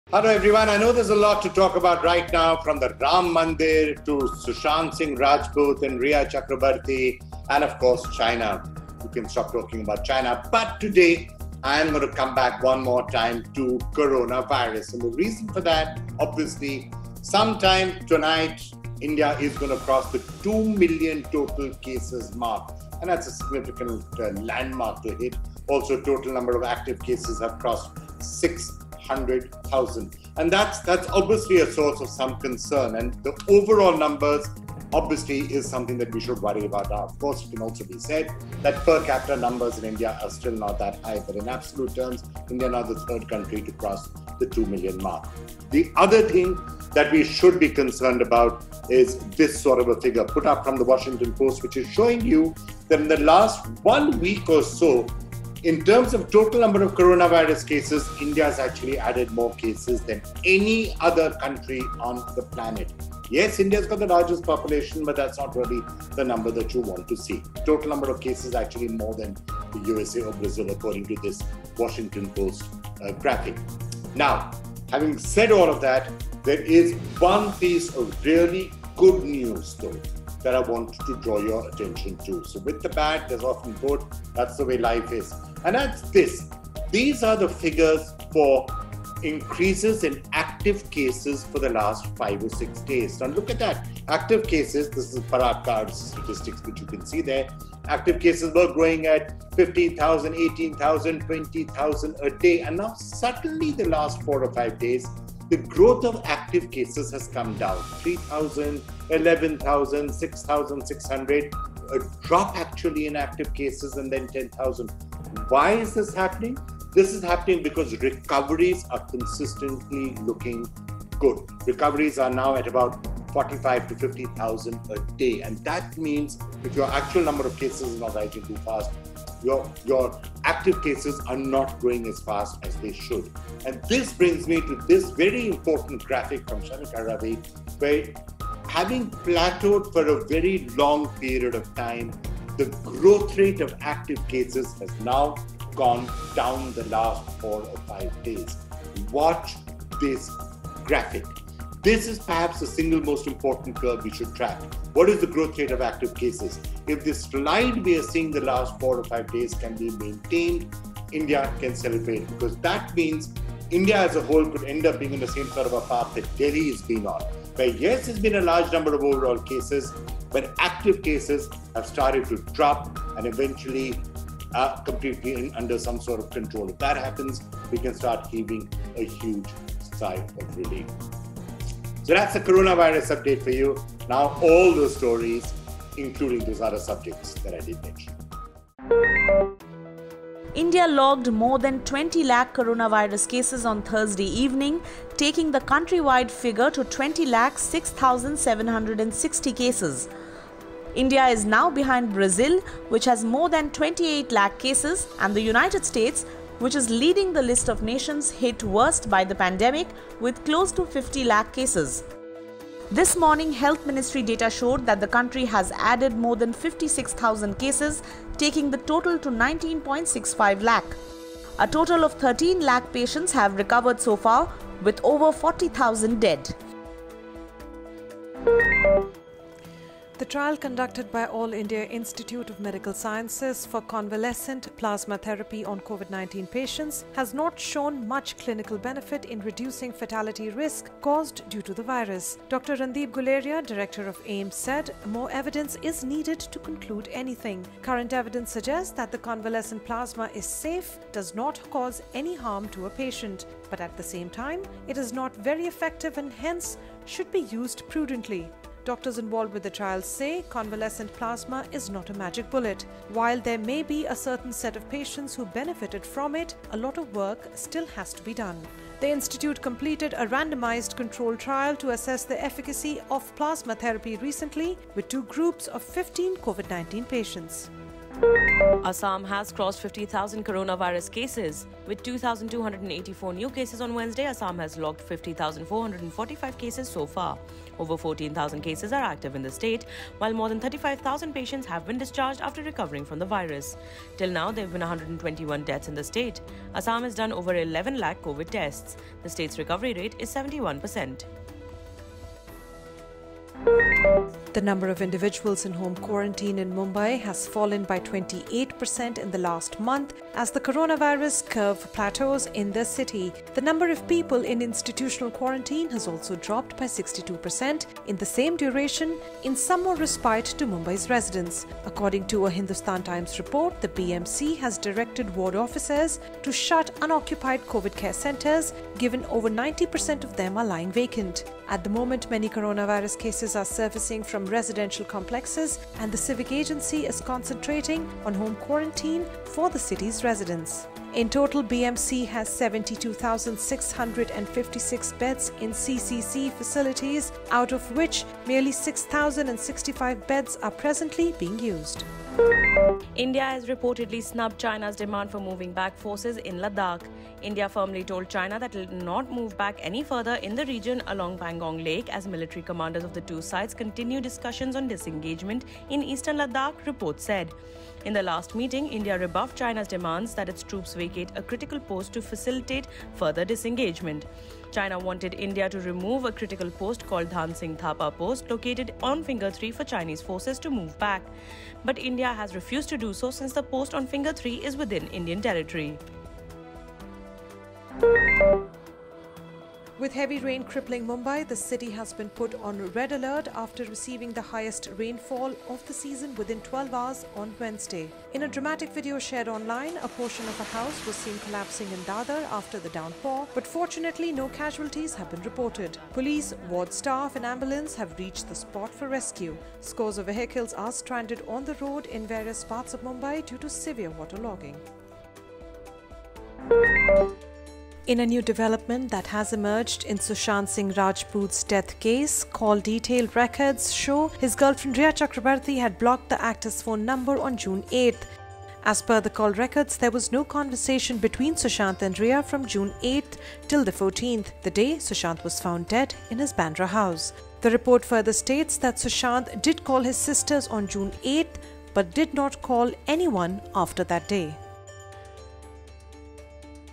India crosses 2 million Covid cases: wrap with Vikram Chandra
India has crossed the 2 million cases mark for Covid-19. Total number of active cases is at 5,95,501 while the death toll is at 40,699. There is however some good news, growth of active cases has come down, this is because recoveries have been good. Having plateaued for a long while, the growth rate of active cases also seems to be on a decline. Could this mean that the spread of the virus is under control? Top stories with Vikram Chandra.